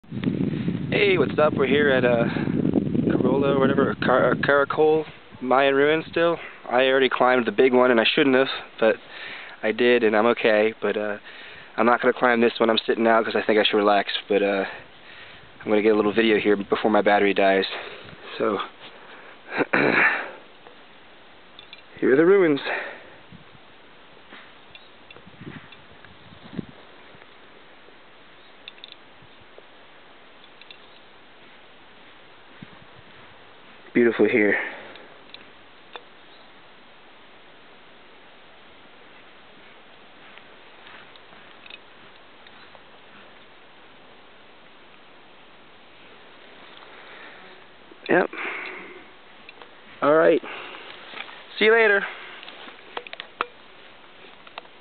Belize: At the Caracol Ruins
Another Panoramic video I took at while visiting the impressive Mayan ruins of Caracol.